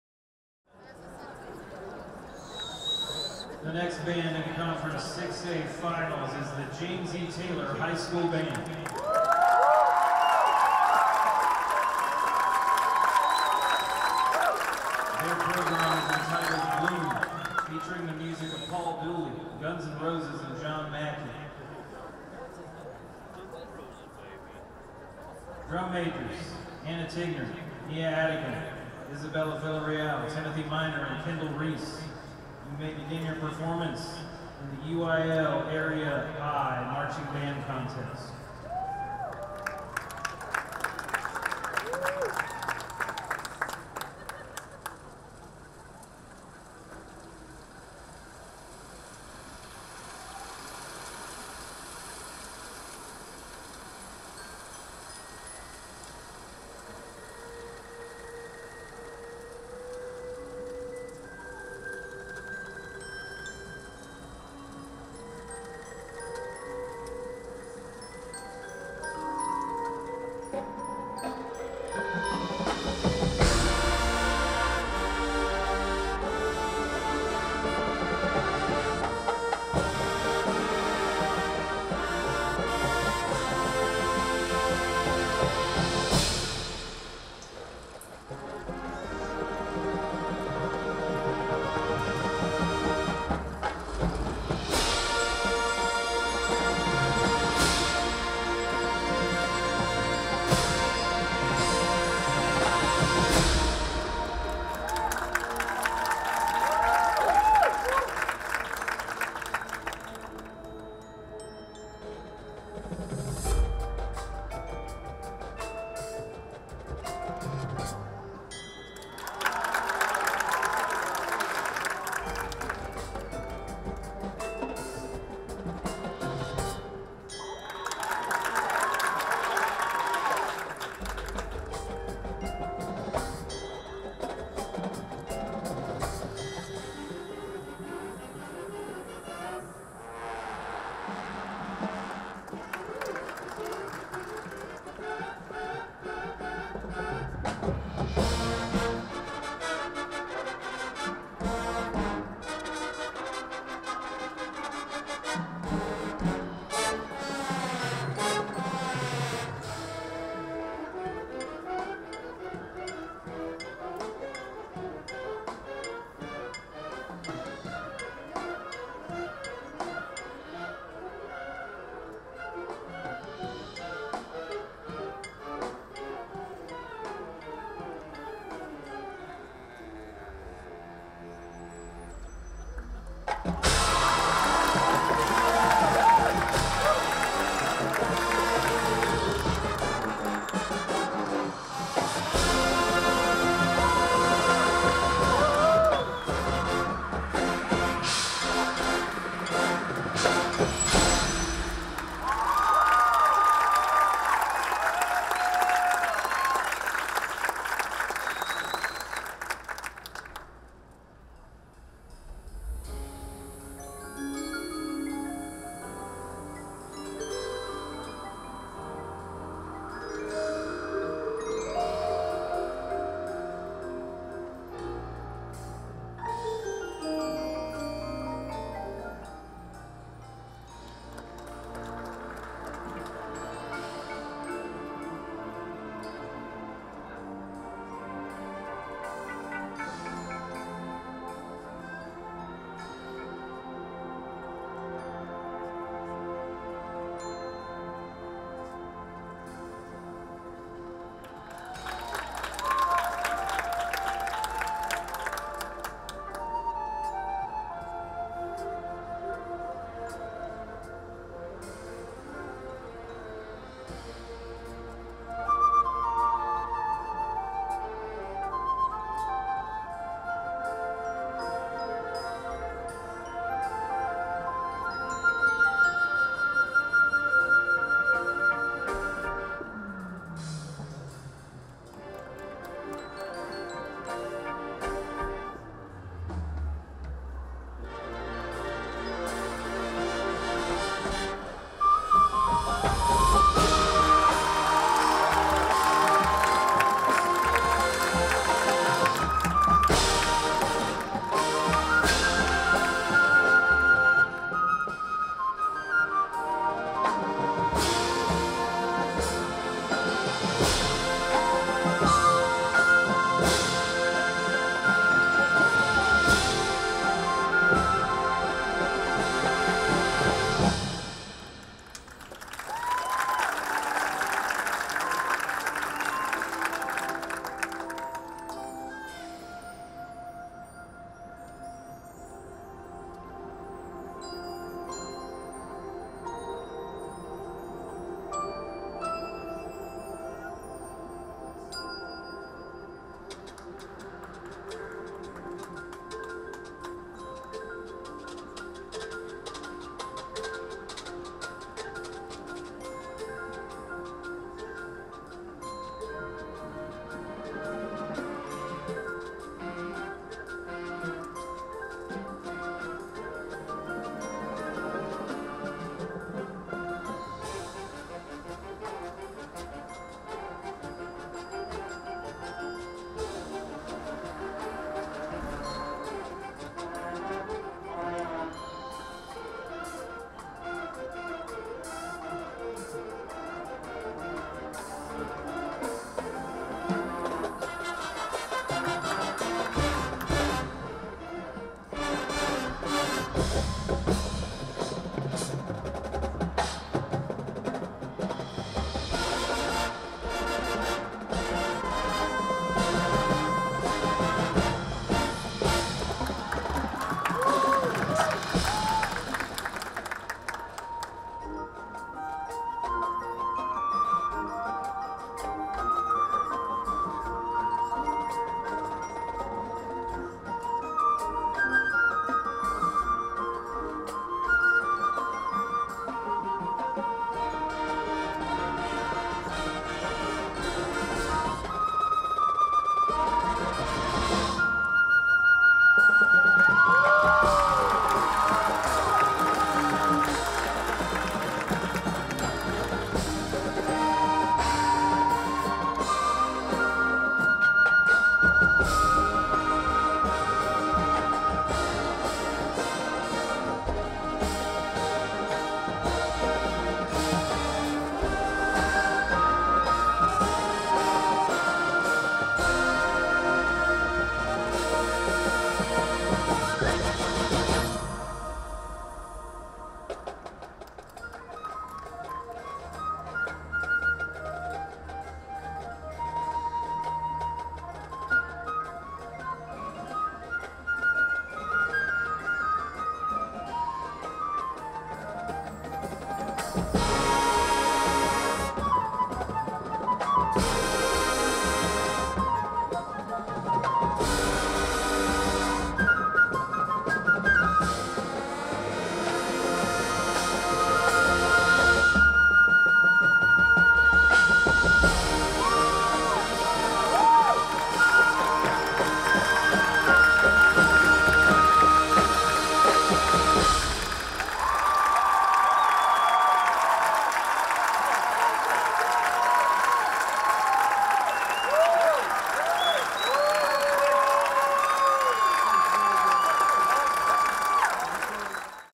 James E. Taylor High School, UIL Area "I" Marching Band Contest
Sixth Place:  James E. Taylor High School, UIL Area "I" Marching Band Contest, Legacy Stadium, Katy, TX, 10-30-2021.